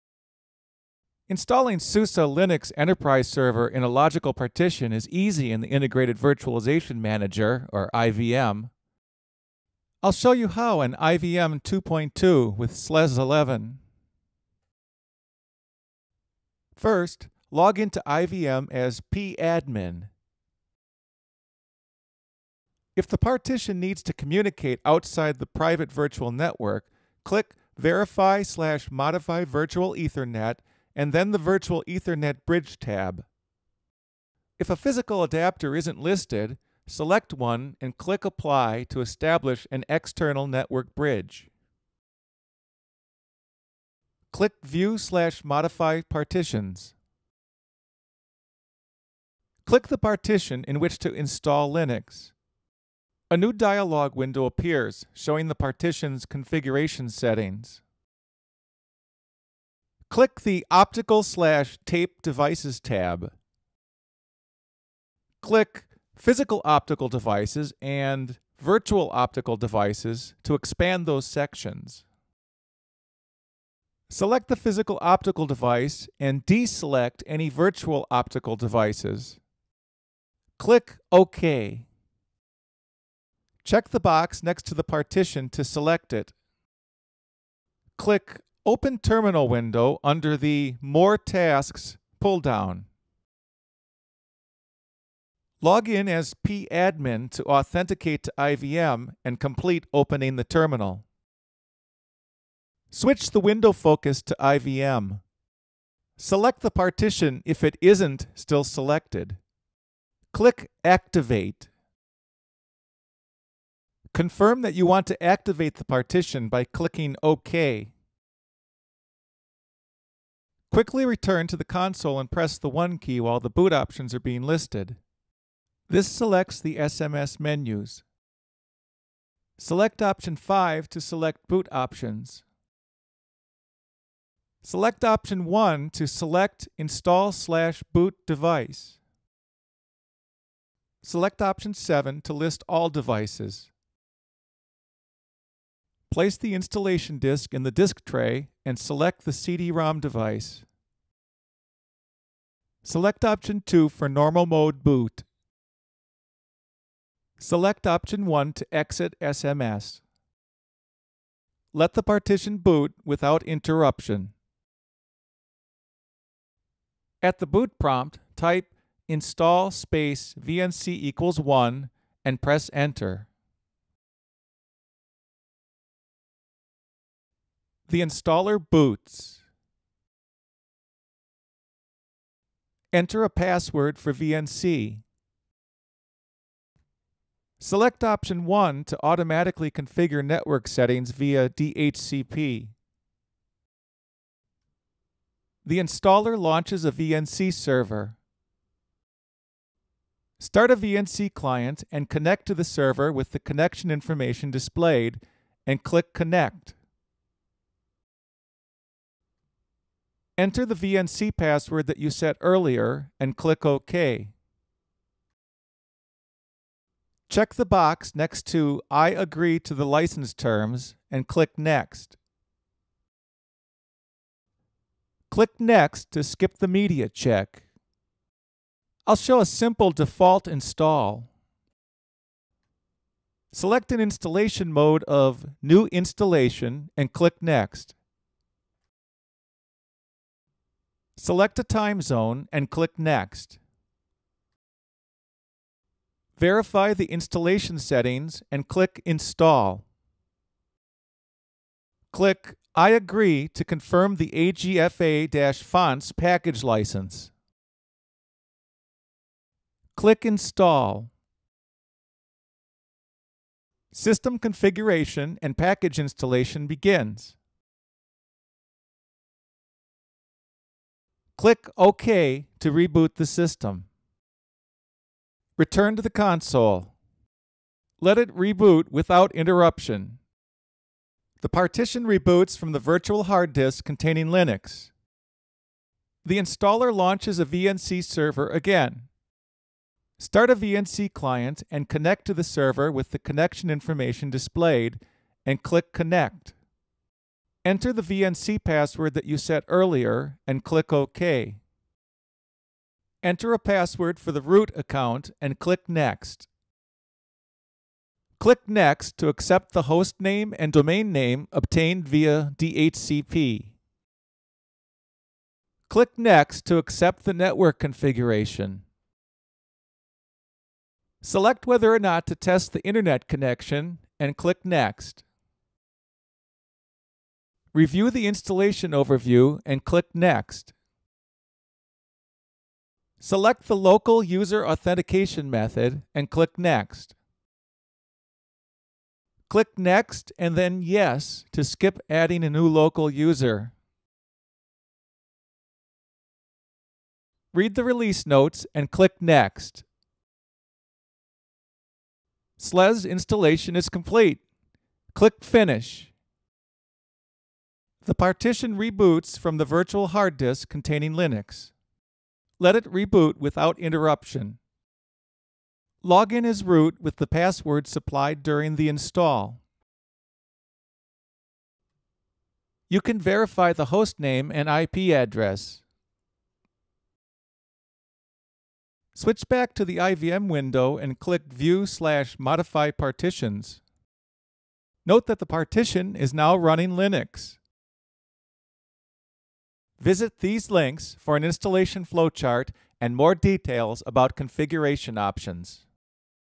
IBM PowerLinux install SuSE Linux Enterprise Server in an LPAR with IVM
This video shows you how to install SuSE Linux Enterprise Server in an LPAR with IVM on an IBM PowerLinux server.